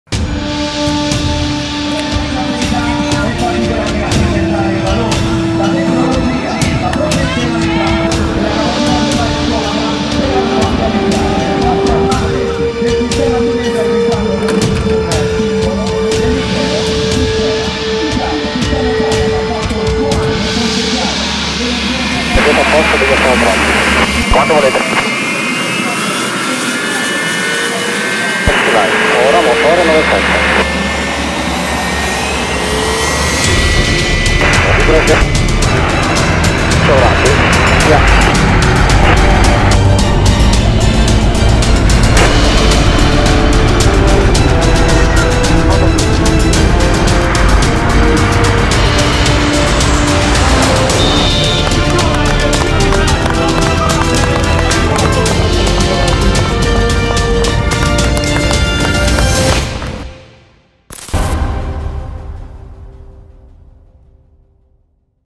Il verde, il bianco e il rosso sulla 15ª tappa della corsa rosa
Aeronautica Militare - Gioco di squadra, tecnologia, addestramento e tanta passione sono solo alcuni degli elementi cardine che accomunano il mondo del ciclismo a quello dell'Aeronautica Militare.
Oggi, per la partenza della 15ª tappa del 103°@Giro d'Italia dal 2° Stormo dell'Aeronautica Militare, le Frecce Tricolori si sono alzate in volo con lo stesso spirito di squadra con il quale qualche mese fa hanno abbracciato simbolicamente l’Italia intera.